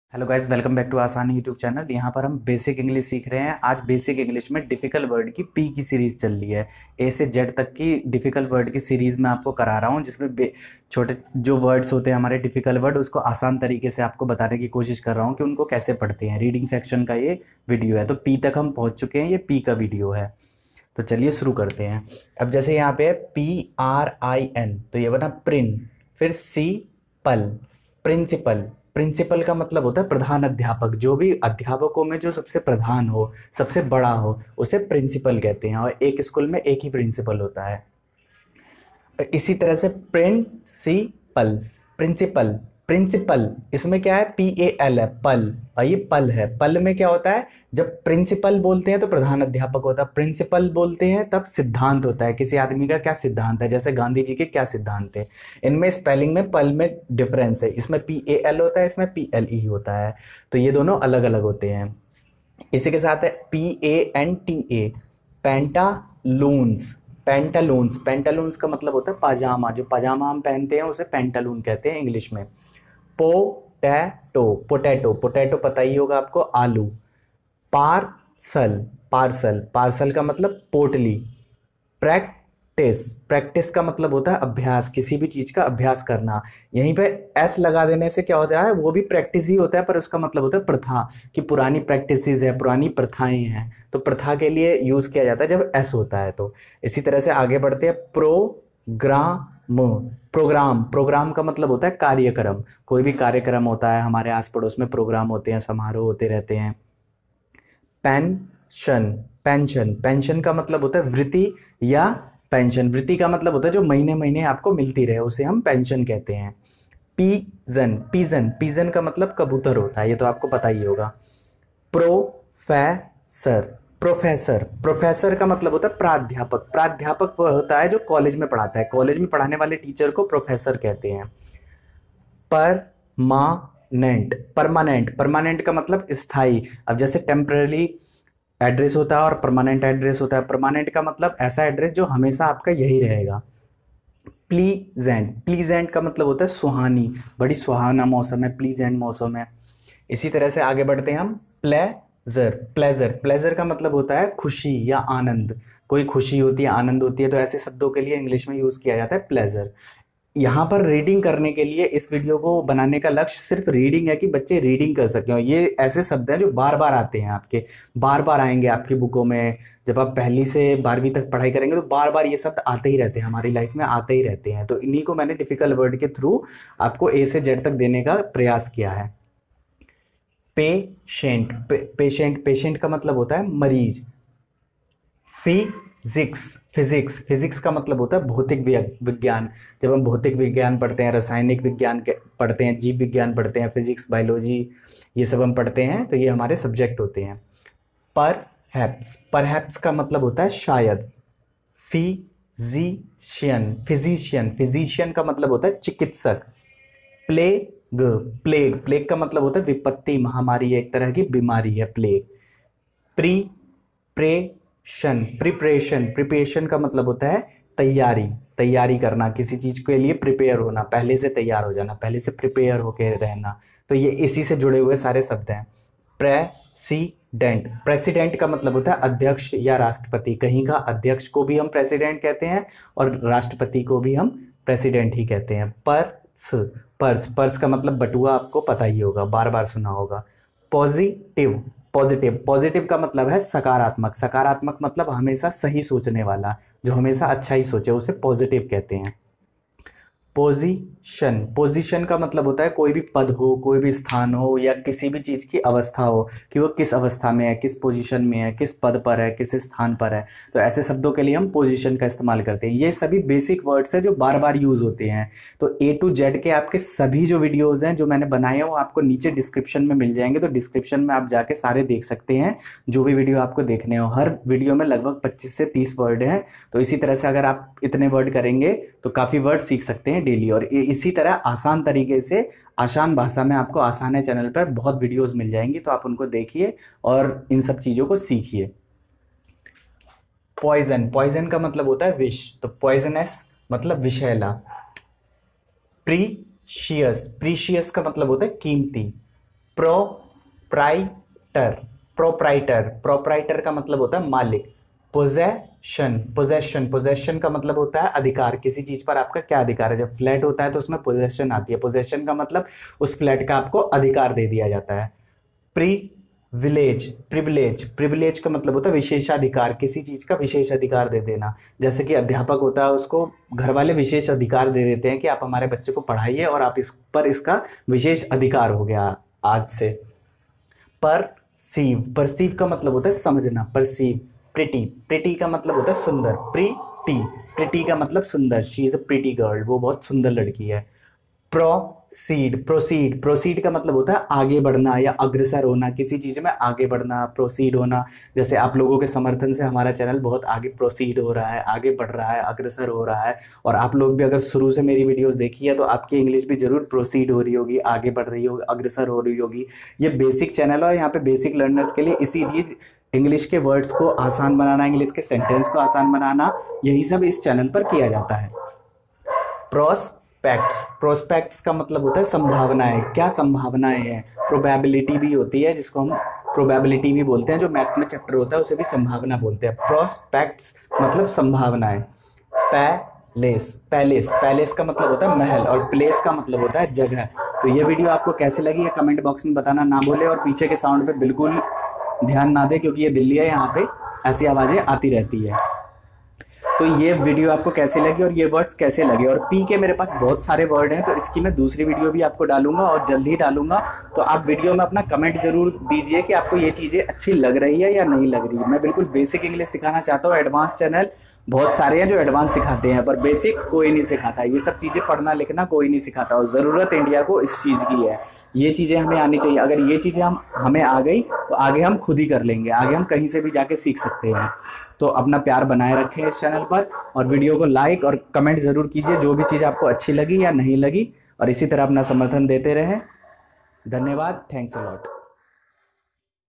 Difficult Words 【Pp】 For Basic Learners By AsaanHai
DIFFICULT WORDS Which is started with letter P...

In this video we learn about some difficult words which is started with the alphabet Pp.

Basic Word Meanings Series Specially For Basic Learners .A To Z .

Difficult Words Pp